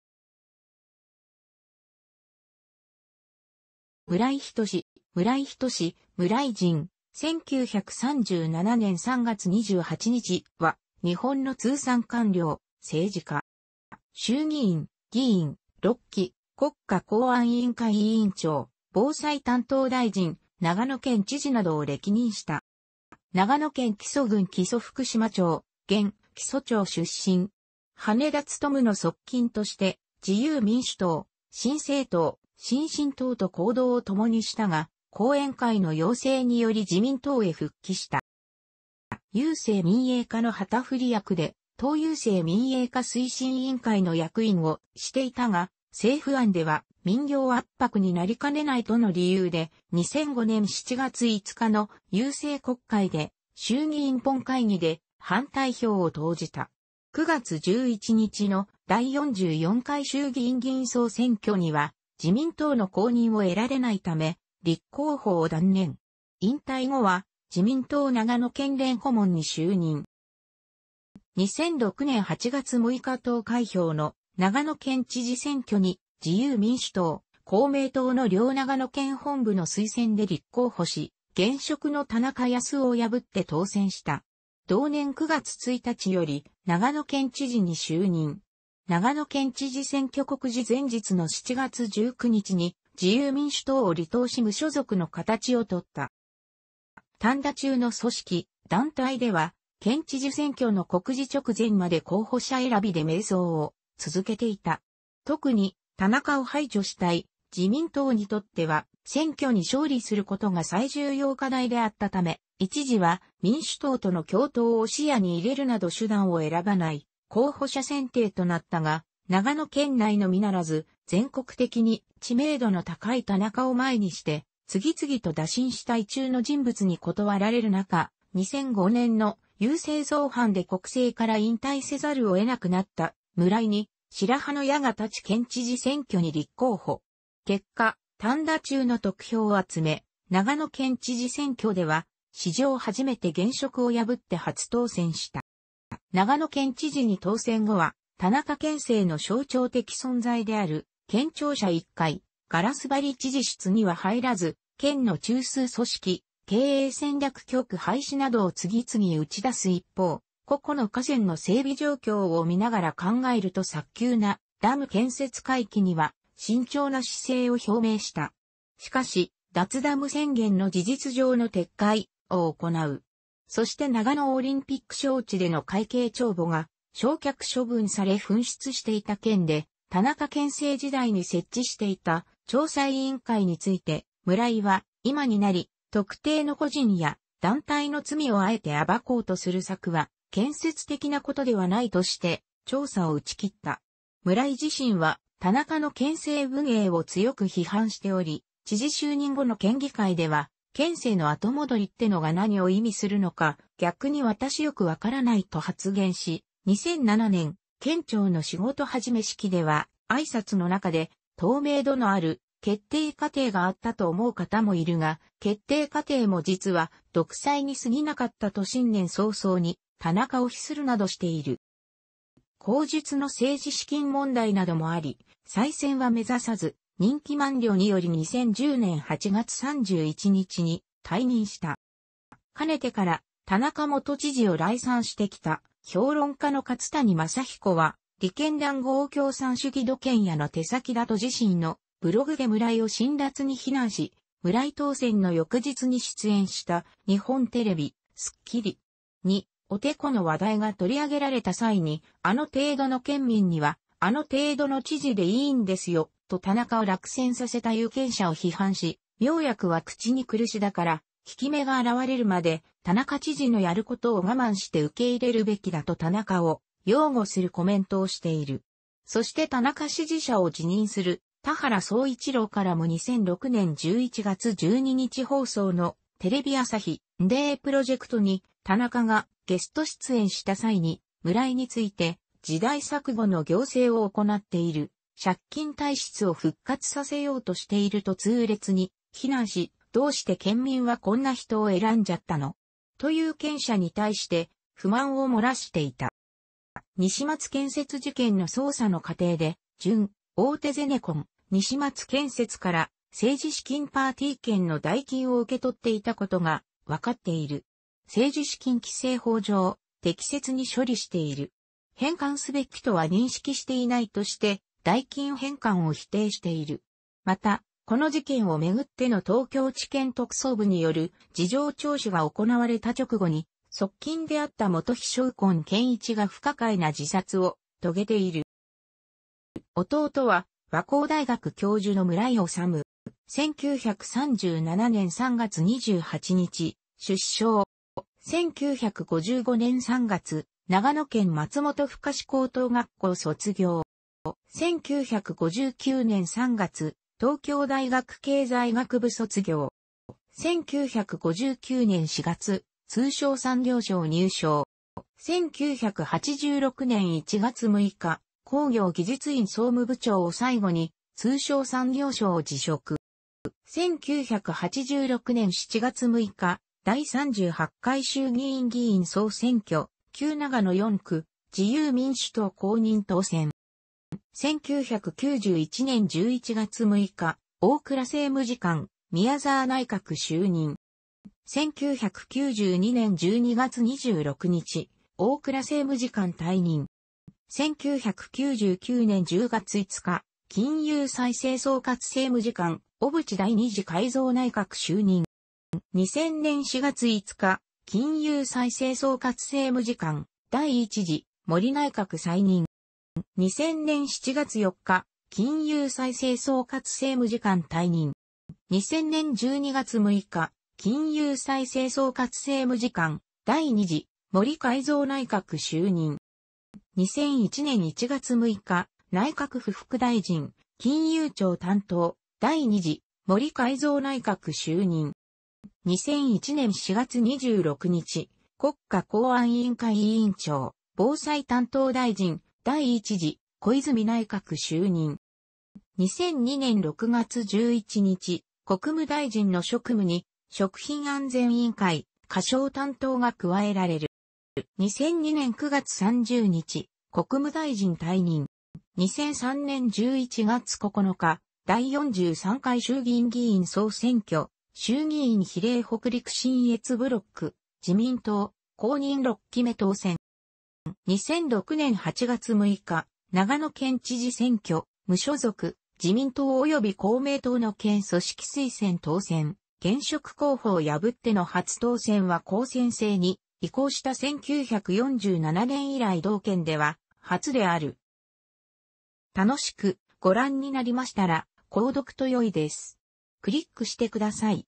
村井仁
村井 仁（むらい じん、1937年3月28日 – ）は、日本の通産官僚、政治家。

衆議院議員（6期）、国家公安委員会委員長、防災担当大臣、長野県知事などを歴任した。

長野県木曽郡木曽福島町（現・木曽町）出身。羽田孜の側近として自由民主党・新生党・新進党と行動をともにしたが、後援会の要請により自民党へ復帰した。

郵政民営化の旗振り役で、党郵政民営化推進委員会の役員をしていたが、「政府案では、民業圧迫になりかねない」との理由で、2005年7月5日の郵政国会で衆議院本会議で反対票を投じた。9月11日の第44回衆議院議員総選挙には、自民党の公認を得られないため、立候補を断念。引退後は自民党長野県連顧問に就任。

2006年8月6日投開票の長野県知事選挙に、自由民主党・公明党の両長野県本部の推薦で立候補し、現職の田中康夫を破って当選した。同年9月1日より長野県知事に就任。長野県知事選挙告示前日の7月19日に自由民主党を離党し無所属の形を取った。

「反田中」の組織・団体では県知事選挙の告示直前まで候補者選びで迷走を続けていた。特に田中を排除したい自民党にとっては選挙に勝利することが最重要課題であった為、一時は民主党との共闘を視野に入れるなど手段を選ばない候補者選定となったが、長野県内のみならず全国的に知名度の高い田中を前にして、次々と打診した意中の人物に断られる中、2005年の郵政造反で国政から引退せざるをえなくなった村井に白羽の矢が立ち県知事選挙に立候補。結果「反田中」の得票を集め、長野県知事選挙では史上初めて現職を破って初当選した。

長野県知事に当選後は、田中県政の象徴的存在である県庁舎1階「ガラス張り知事室」には入らず、県の中枢組織「経営戦略局」廃止などを次々打ち出す一方、“個々の河川の整備状況を見ながら考える”と早急なダム建設回帰には慎重な姿勢を表明した。しかし、「脱ダム宣言」の事実上の撤回、を行う。そして長野オリンピック招致での会計帳簿が焼却処分され紛失していた件で、田中県政時代に設置していた調査委員会について、村井は「今になり特定の個人や団体の罪をあえて暴こうとする作業は建設的なことではない」として調査を打ち切った。村井自身は田中の県政運営を強く批判しており、知事就任後の県議会では「県政の後戻りってのが何を意味するのか、逆に私良く分からない」と発言し、2007年、県庁の仕事始め式では、挨拶の中で「透明度のある決定過程があったと思う方もいるが、決定過程も実は独裁に過ぎなかった」と新年早々に田中を批判するなどしている。

後述の政治資金問題などもあり再選は目指さず、任期満了により2010年8月31日に退任した。

かねてから田中元知事を礼賛してきた評論家の勝谷誠彦は、「利権談合共産主義土建屋の手先だ」と自身のブログで村井を辛辣に非難し、村井当選の翌日に出演した日本テレビ「スッキリ!!」においてこの話題が取り上げられた際に「あの程度の県民にはあの程度の知事でいいんですよ」と田中を落選させた有権者を批判し、「妙薬は口に苦しだから、効き目が現れるまで田中知事のやることを我慢して受け入れるべきだ」と田中を擁護するコメントをしている。そして田中支持者を自認する田原総一朗からも2006年11月12日放送のテレビ朝日「サンデープロジェクト」に田中がゲスト出演した際に、村井について「時代錯誤の行政を行っている」「借金体質を復活させようとしている」と痛烈に非難し、「どうして県民はこんな人を選んじゃったの?」と有権者に対して不満を漏らしていた。

西松建設事件の捜査の過程で、準大手ゼネコン西松建設から政治資金パーティー券の代金を受け取っていたことがわかっている。「政治資金規正法上、適切に処理している。返還すべきとは認識していない」として、代金返還を否定している。また、この事件を巡っての東京地検特捜部による事情聴取が行われた直後に、側近であった元秘書右近謙一が不可解な自殺を遂げている。

弟は和光大学教授の村井紀。

1937年3月28日：出生。

1955年3月：長野県松本深志高等学校卒業。

1959年3月：東京大学経済学部卒業。

1959年4月：通商産業省入省。

1986年1月6日：工業技術院総務部長を最後に、通商産業省を辞職。

1986年7...